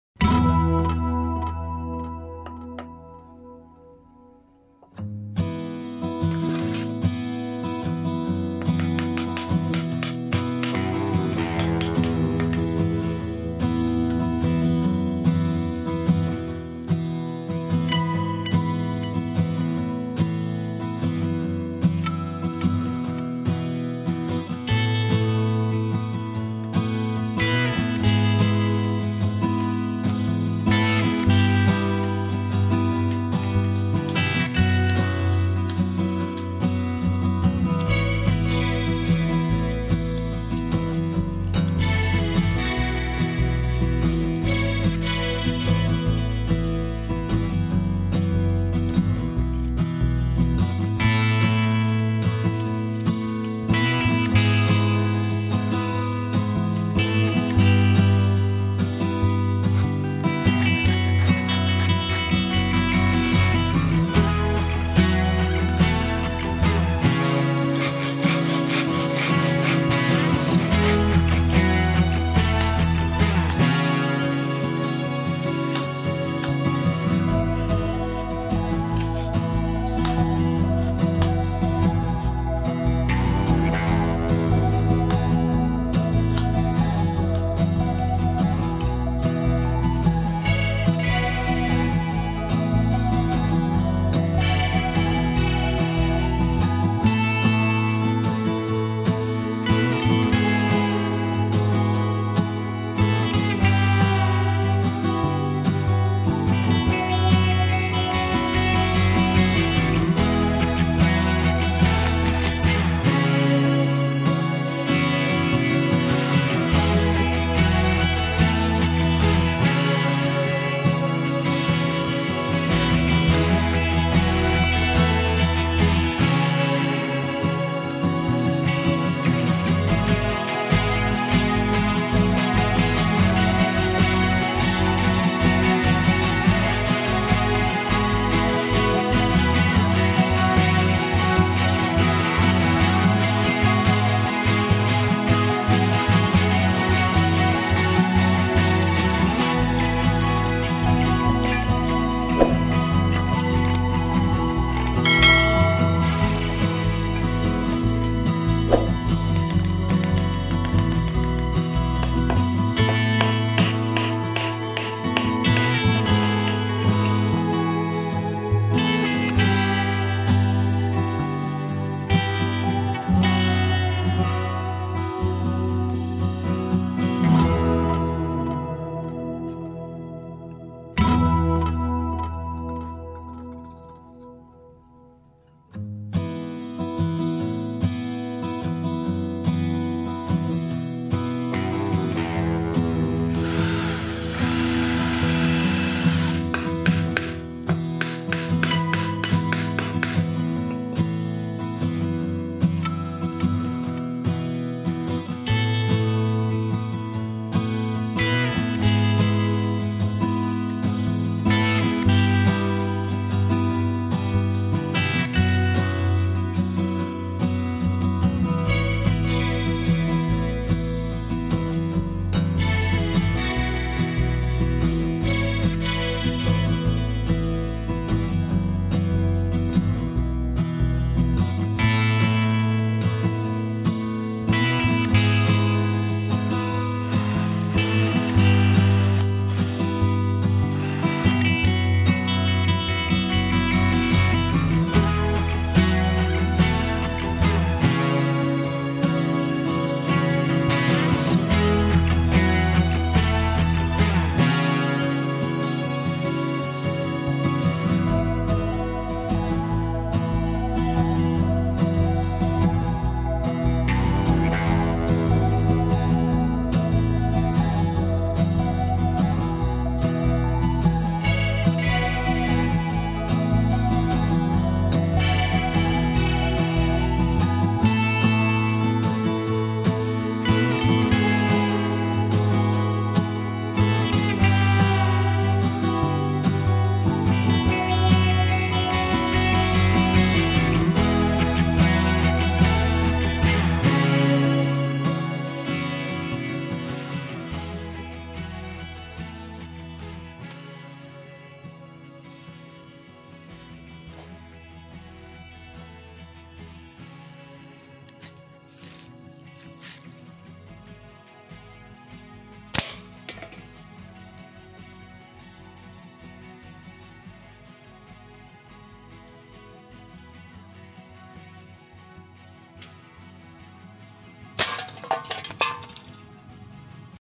DIY Bamboo slingshot | Ketapel unik dan kuat dari bambu
Video ini adalah cara membuat Ketapel unik dan kuat dari bambu,ketapel tradisional ini sangat mudah membuatnya